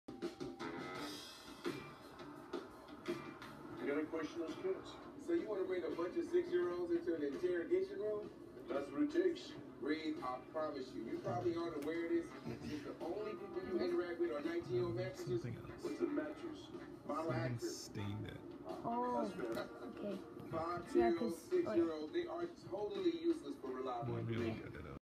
During movie kitten gets back row seats!
She is on the top of the couch during the movie just wondering around she wasn't even scared ONLY 8 WEEKS OLD!